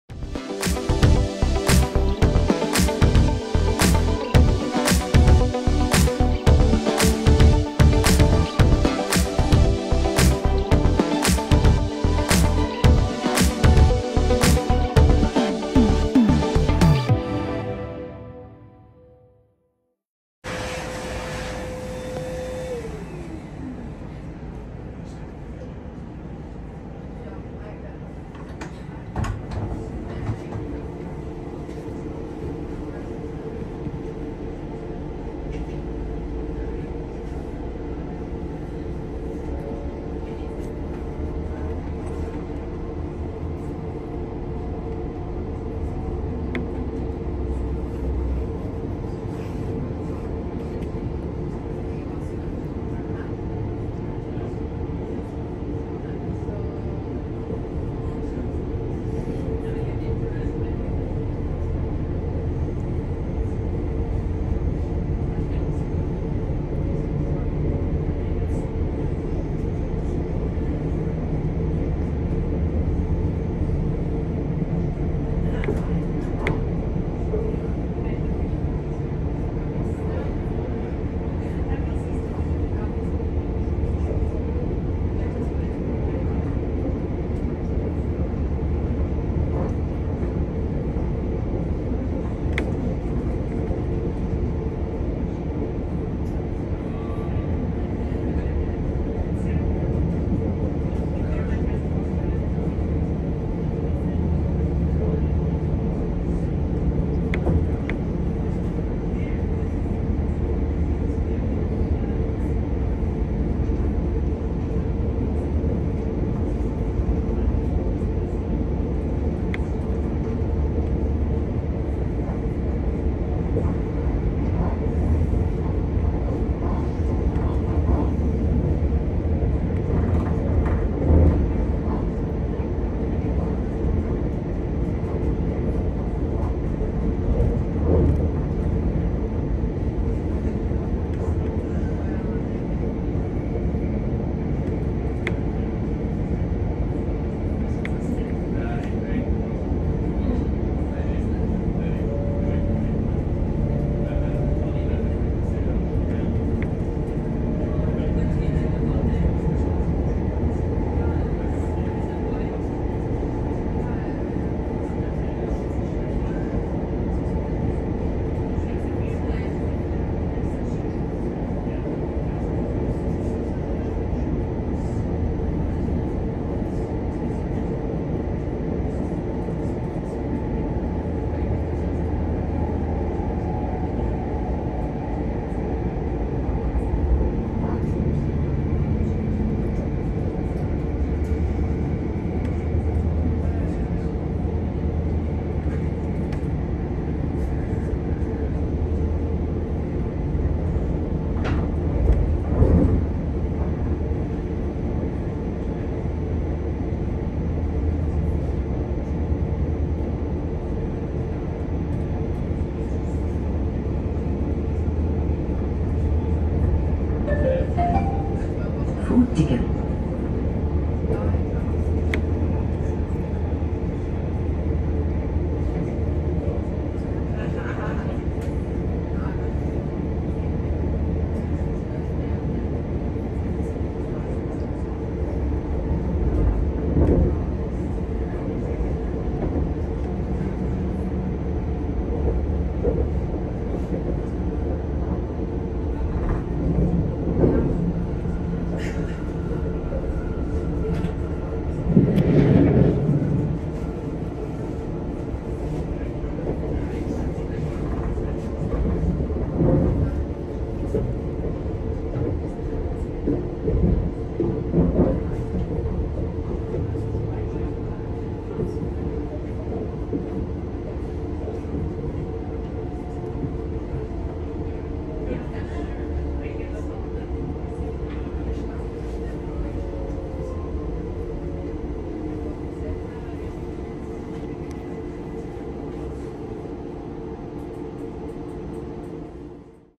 RE1 Mitfahrt: Reichenbach im Kandertal-Frutigen (01.05.2024)
In diesem Video seht ihr meine Mitfahrt im RE1 von Reichenbach im Kandertal nach Frutigen.

Ich wünsche euch viel Freude mit diesem Video!